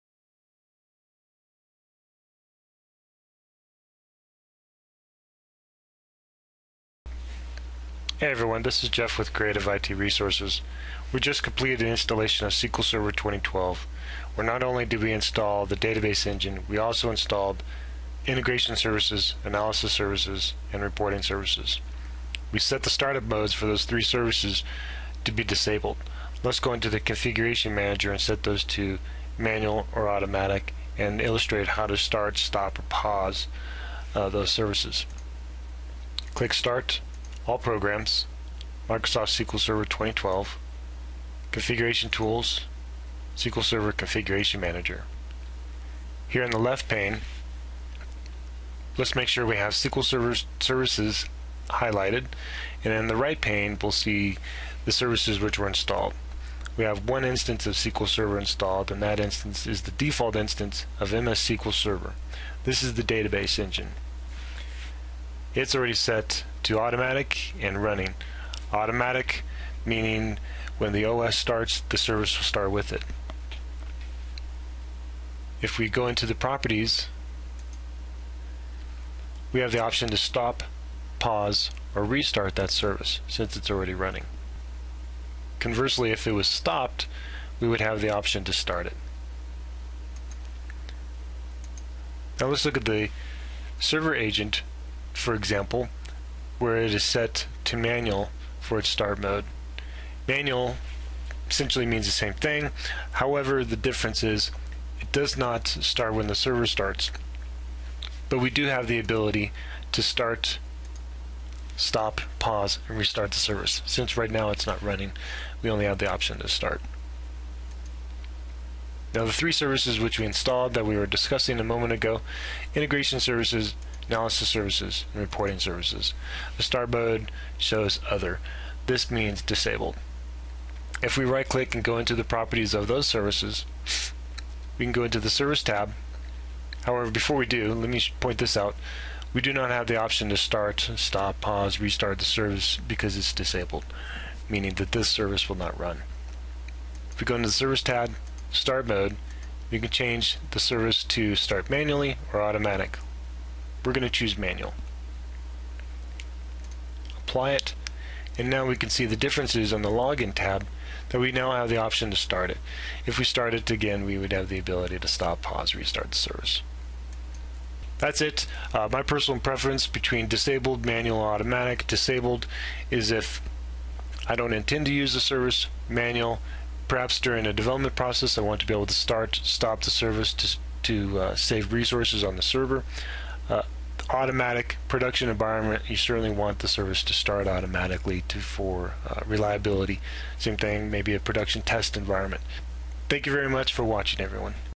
SQL Server 2012 Tutorial - Change SQL Server Service Start Mode
Change a SQL Server Service Start Mode and Start, Stop, Pause or Restart a SQL Server Service.